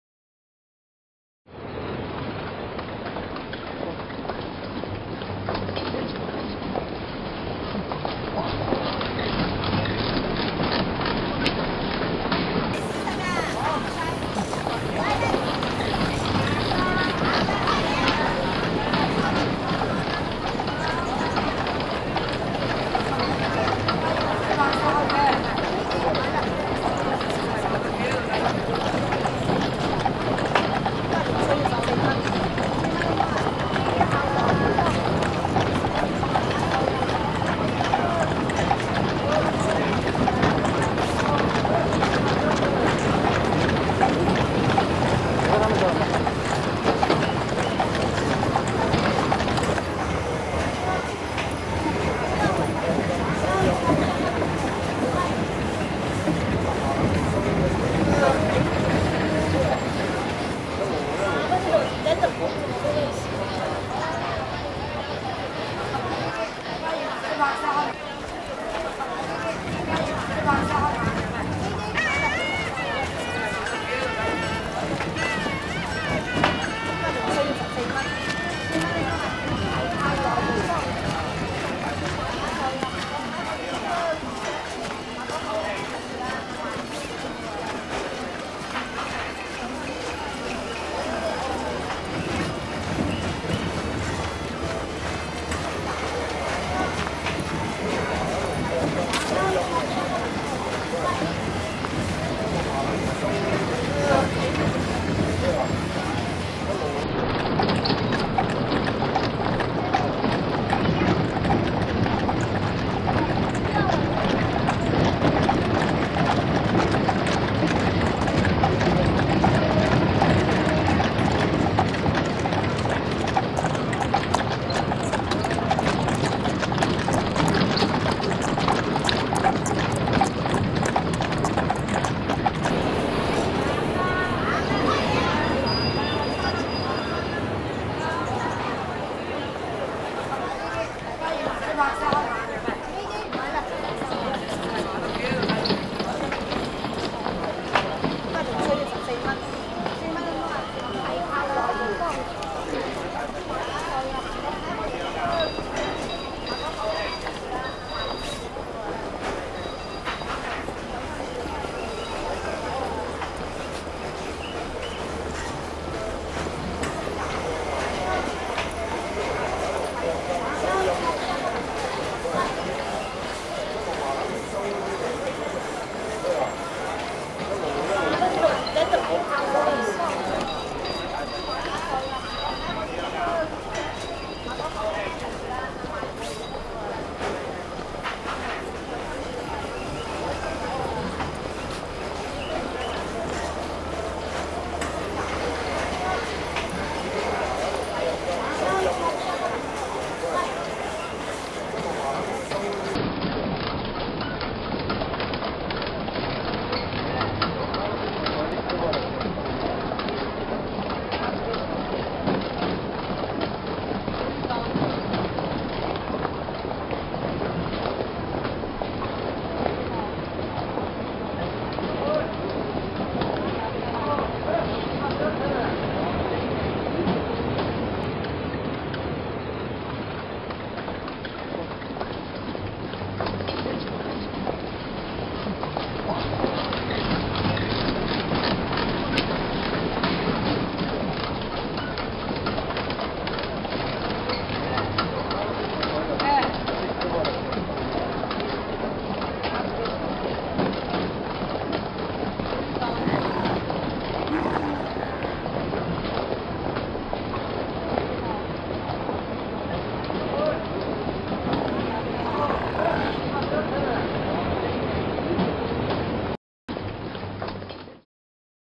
China between 1910-1920 (Beijing - Peking)【4K 60Fps Colorized Stabilized】with Sound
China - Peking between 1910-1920

Upscale (AI)
Frame-Interpolation 60FPS (AI)
Coloring (AI)